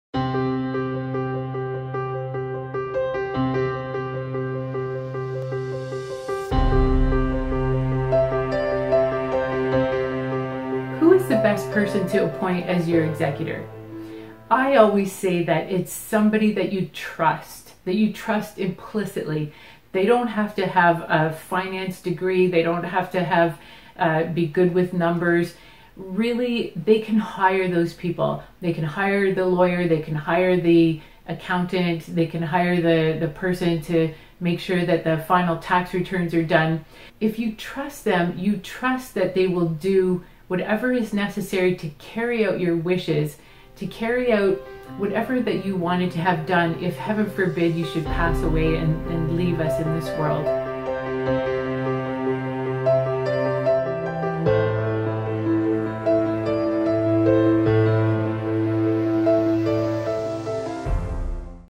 How to choose an executor?
Who is the best person to be your executor?  How to know who to select as your executor. Watch this short video and Maureen will explain.

You know you need a will, let's get you started!

Take our 5 question quiz to find out your risk level of not having a will.